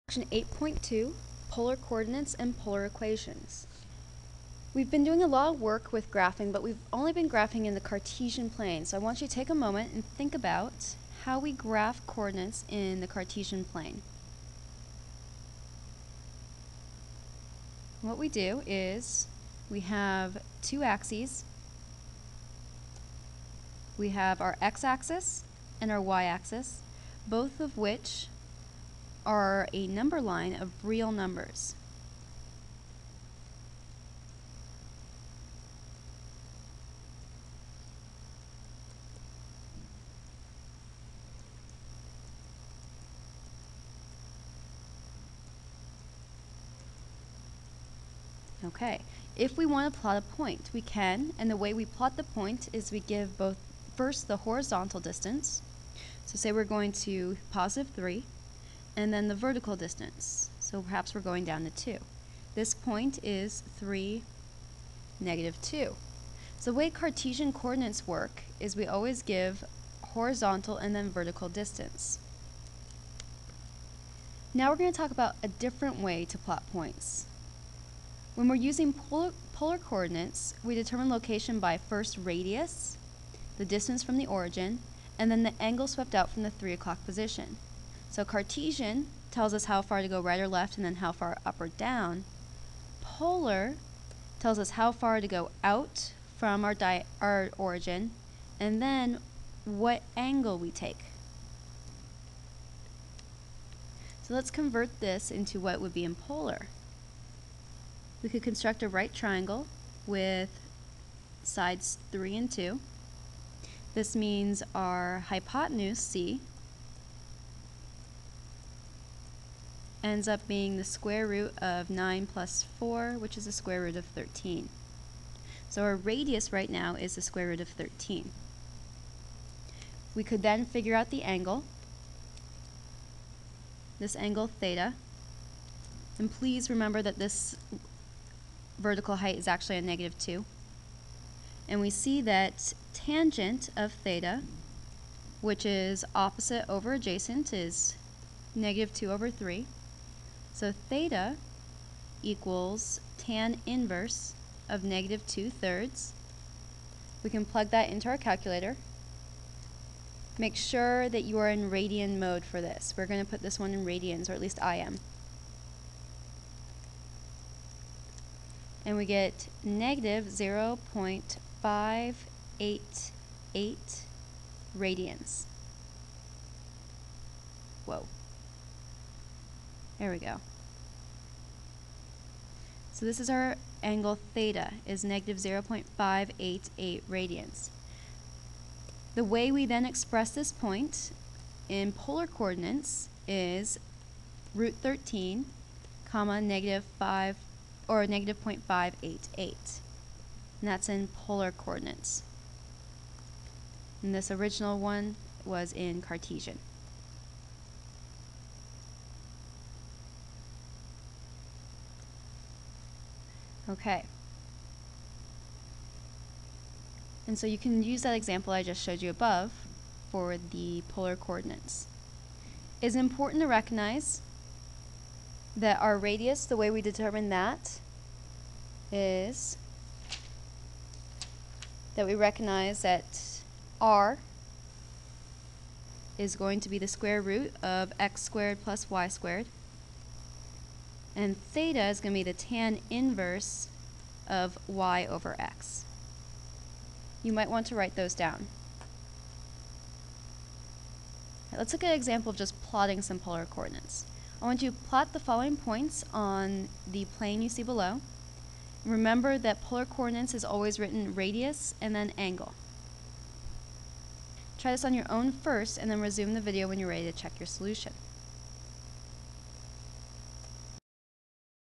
8.2 (1 of 5) Polar Coordinates, Basics.mp4
Polar Coordinates. Basics of polar coordinates, with brief review of Cartesian (rectangular) coordinates.